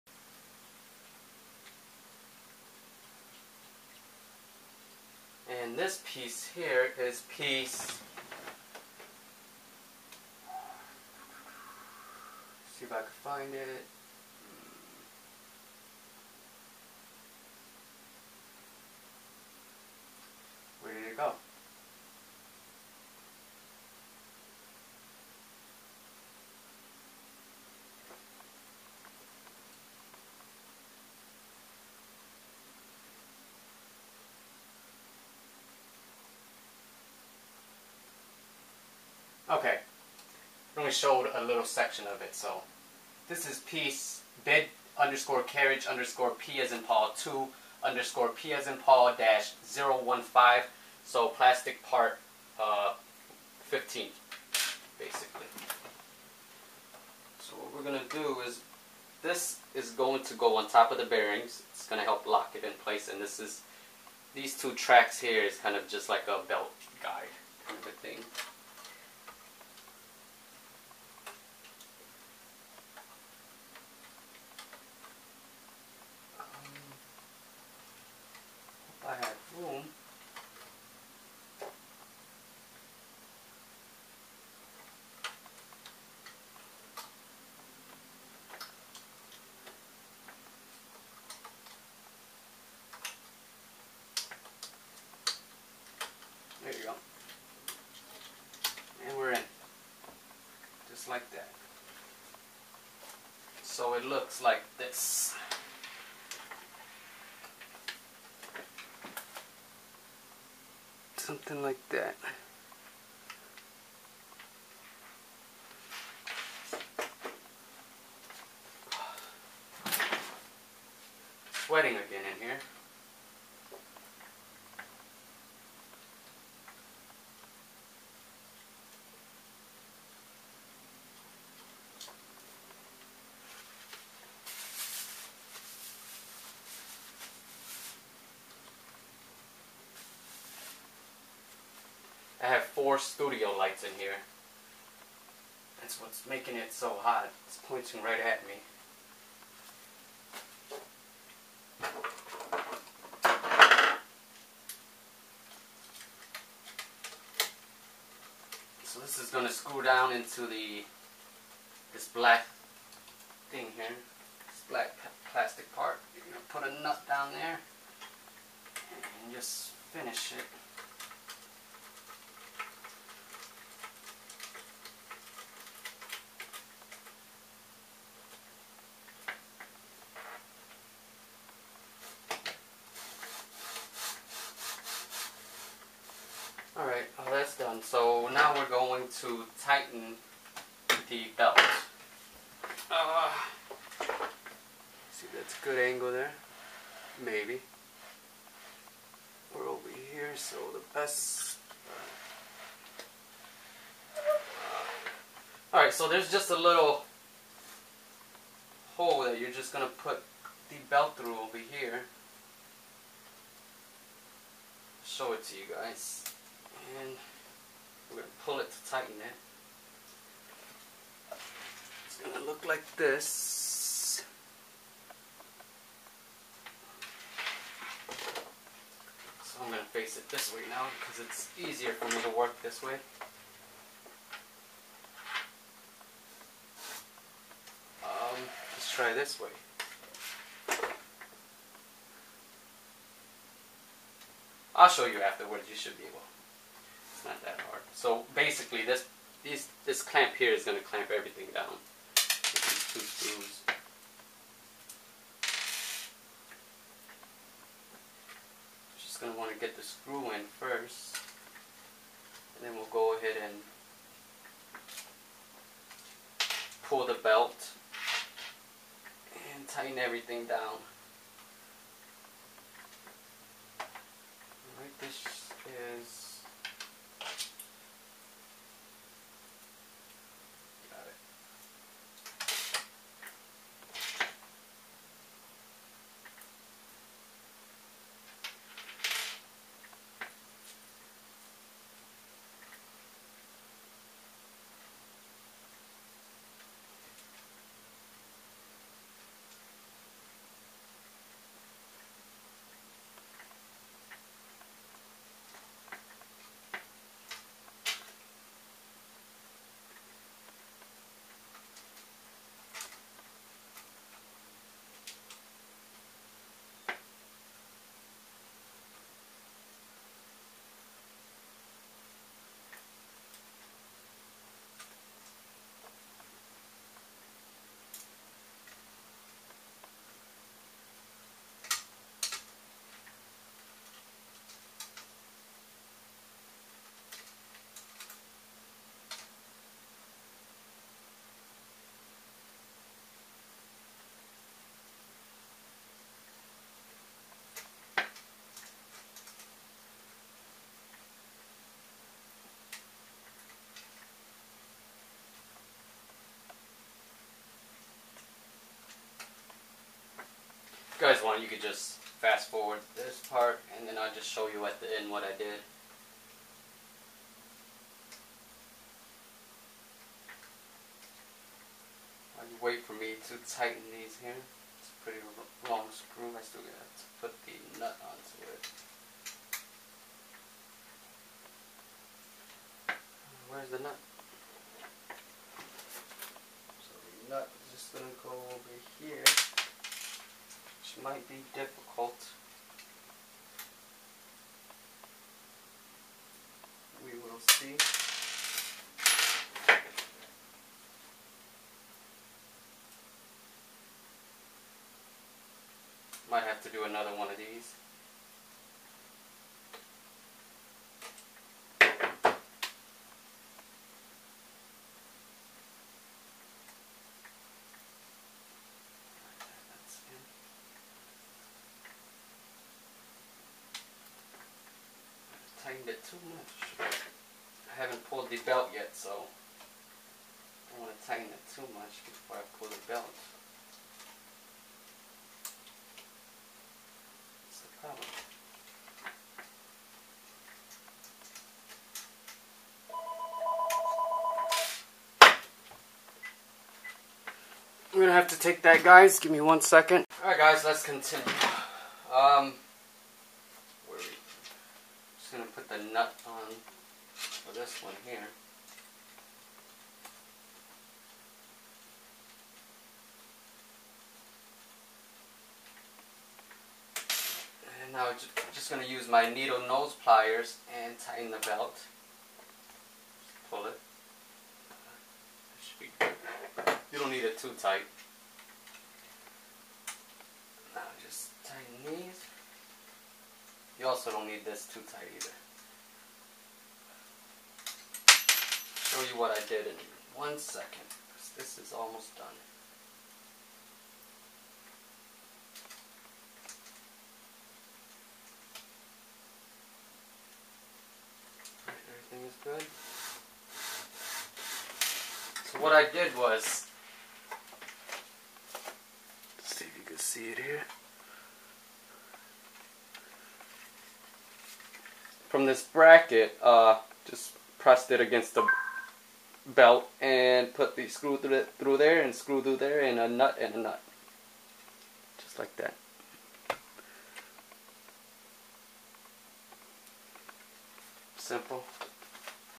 Assembly of Eventorbot. Y axis tutorial part 7
7th video on the assembly of the Eventorbot.

Eventorbot! Open source 3D printer.

For those who do not have access to a 3D printer. You can order the plastic parts and frame